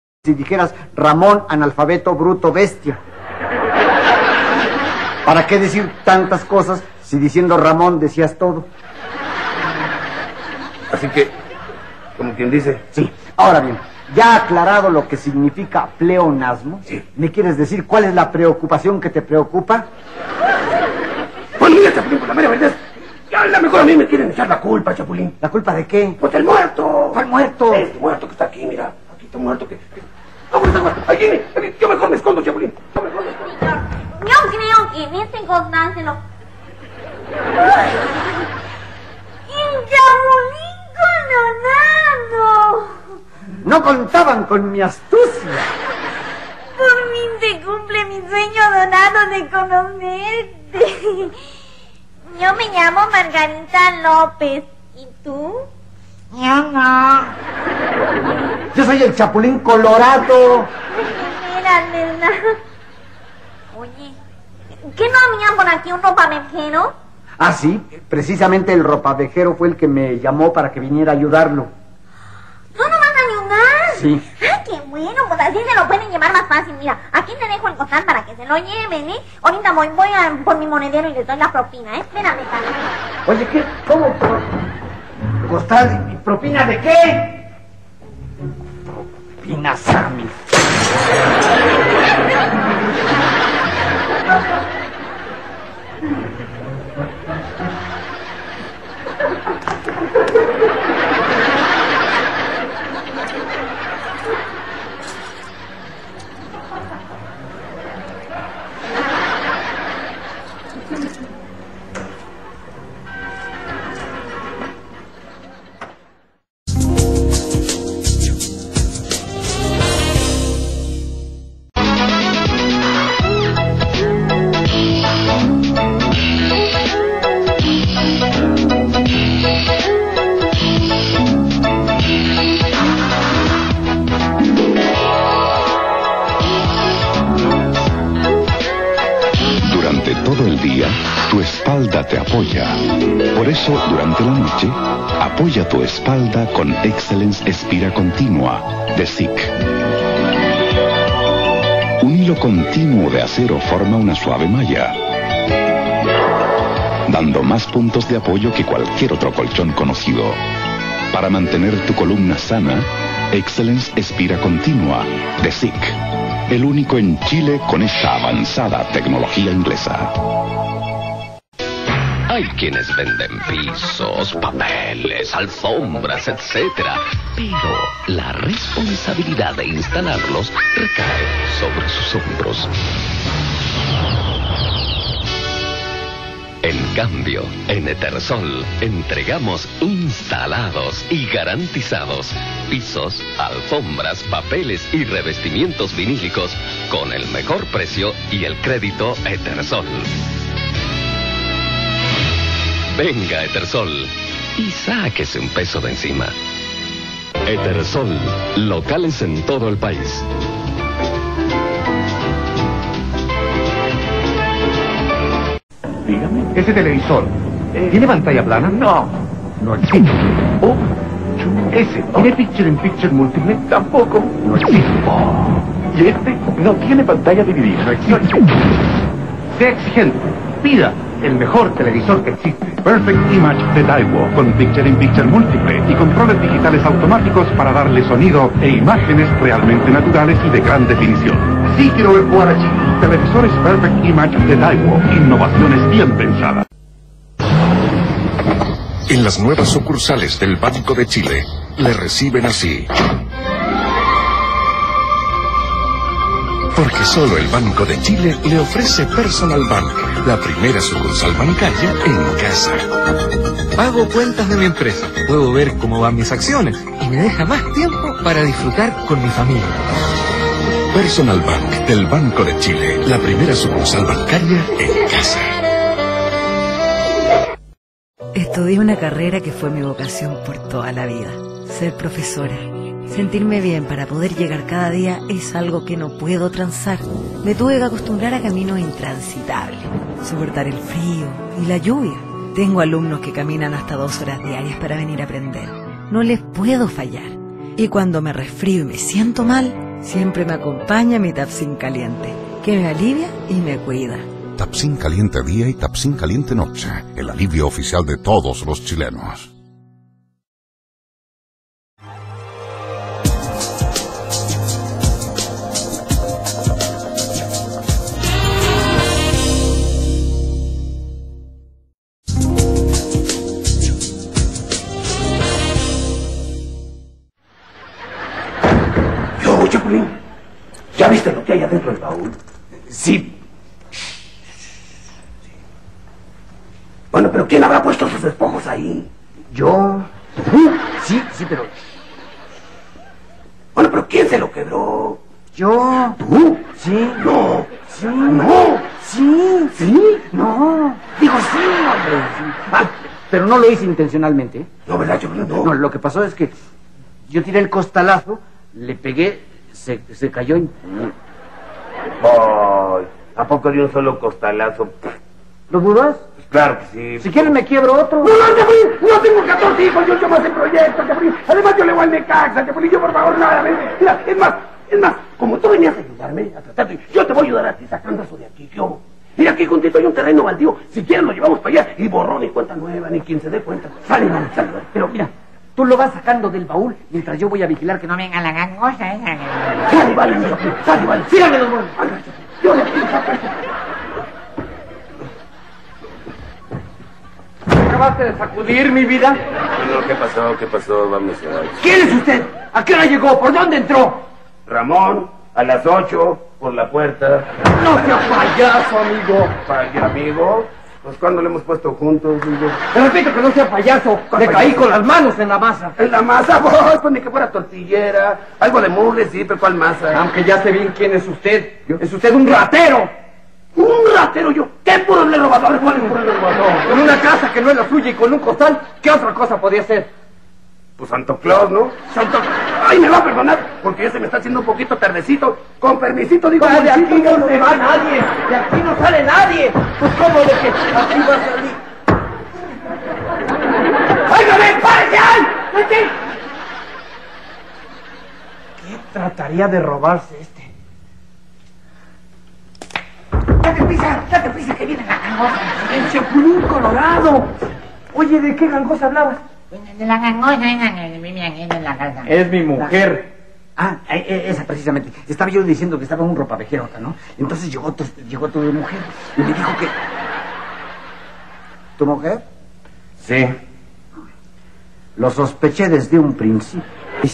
90ª Tanda de Canal 4
-ENTRADA A COMERCIALES (*)
-COMERCIAL COLCHONES EXCELLENCE DE CIC (1991)
-COMERCIAL ETERSOL (1991)
-COMERCIAL TELEVISORES PERFECT IMAGE DE DAEWOO (1998)
-COMERCIAL BANCO DE CHILE (1995)
-COMERCIAL TAPSIN CALIENTE (2009)
-GENÉRICO CANAL 4 (*)
-RETORNO AL PROGRAMA (*)